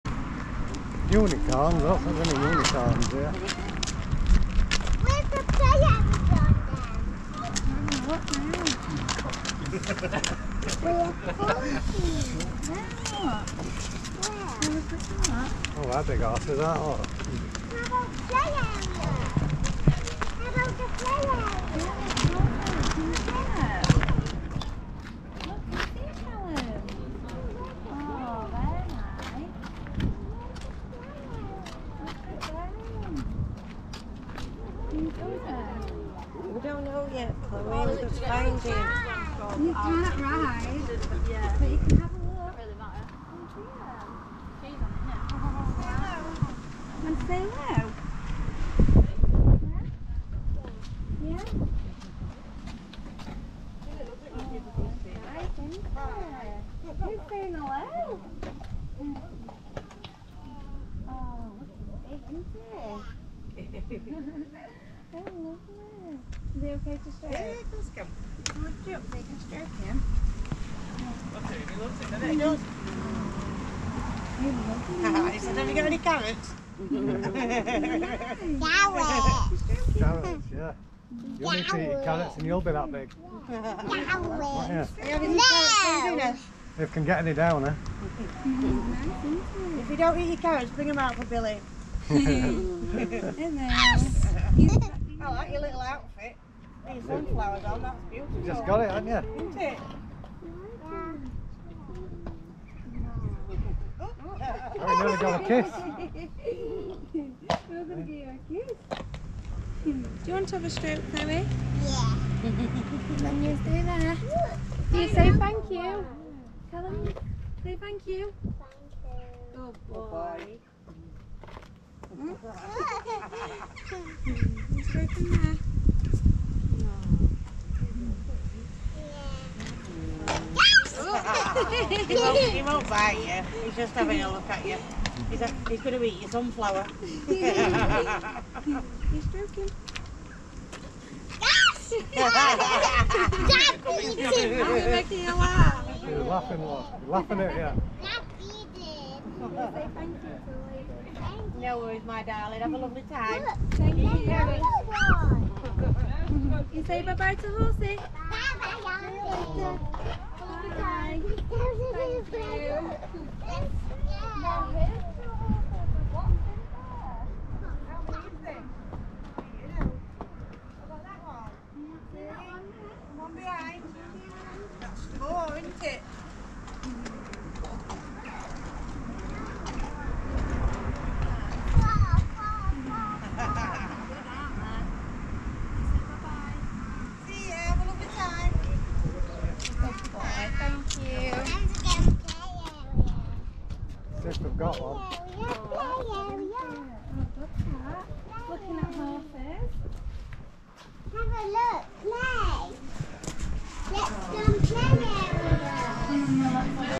360 VR - Stroking a Horse at The Hatfield Chase in Doncaster
This video is best viewed in a VR HEADSET and ensure your video quality is set to the highest, if viewing by PC/Laptop then use your mouse to look around.